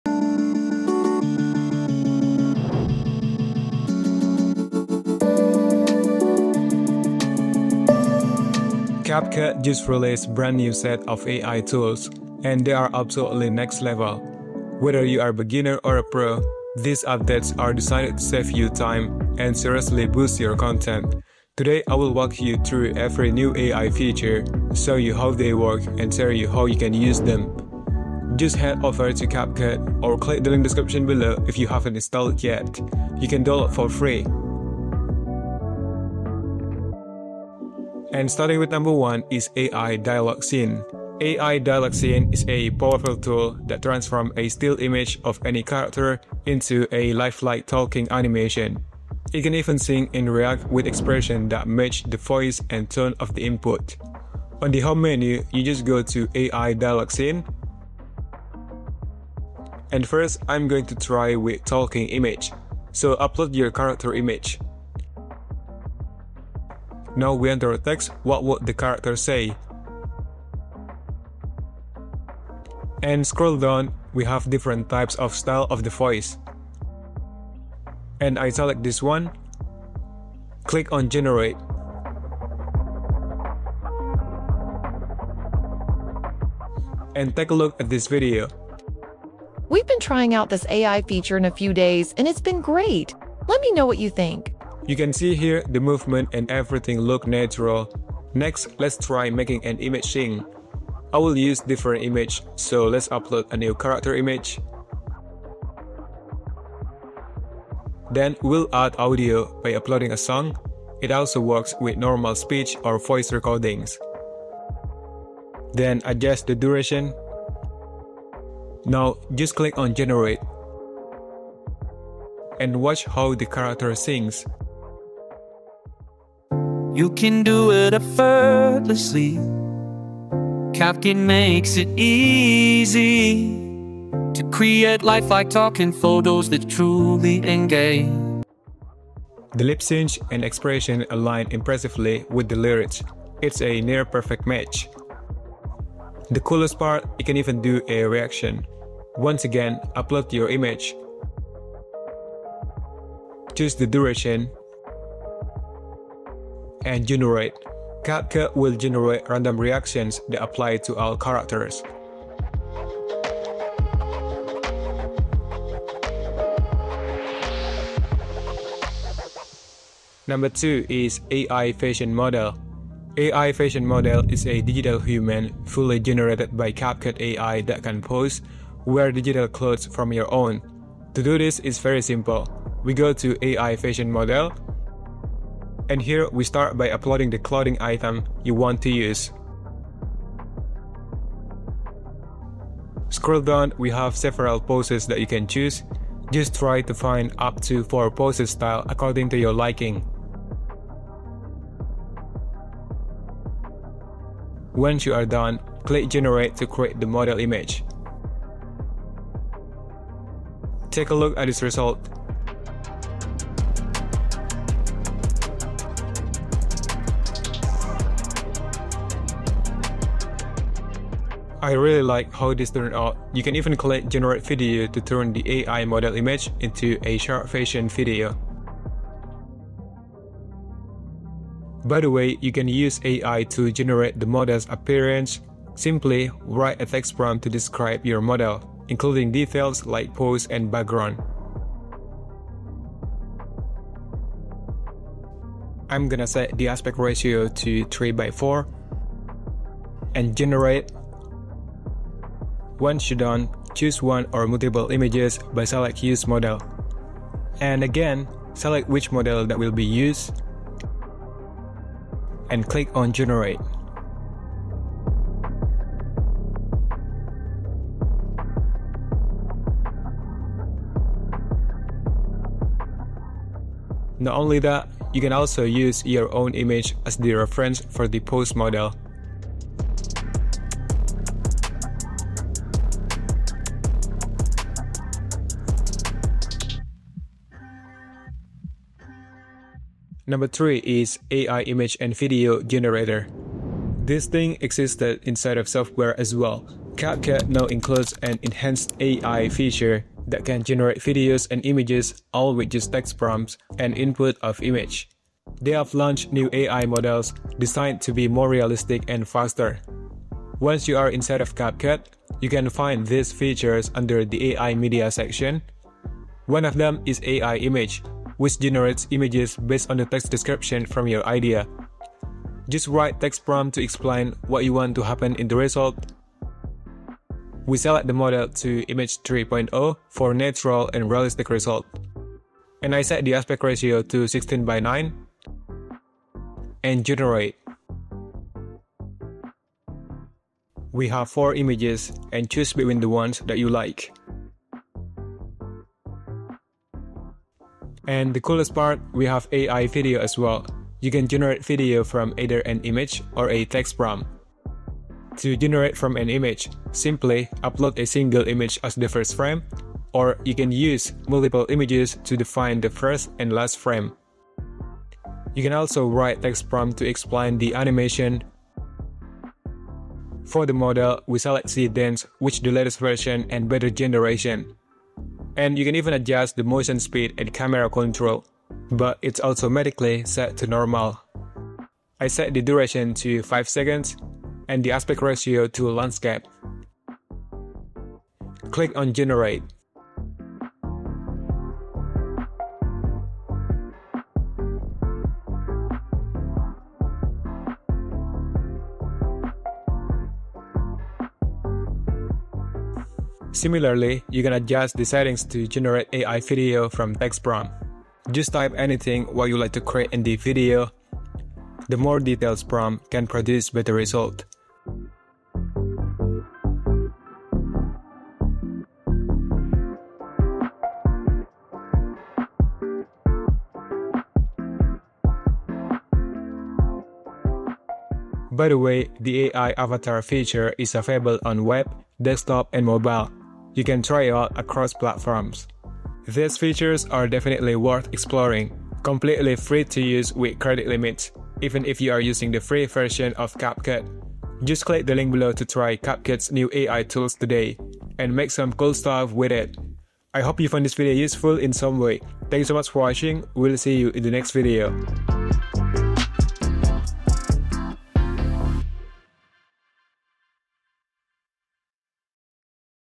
CapCut’s new AI Video Tool are Getting Wild (2025 Update)
CapCut just dropped THREE mind-blowing AI tools that are changing the game for creators:

- Turn text prompts into stunning visuals! Create cinematic videos, stylized images, and more—instantly.

- Generate realistic character conversations with voiceovers, subtitles, and background scenes. Perfect for storytelling, skits, or explainer content!

- Design and generate fashion models in various outfits and poses for virtual catalogs, social media, and e-commerce.

Whether you're a filmmaker, content creator, or just love experimenting with AI tools, this update is a must-see.

0:00 Intro
0:38 Ai Dialogue Scene
2:50 Ai Fashion Model
4:50 Ai Image & Video Generator
7:06 Outro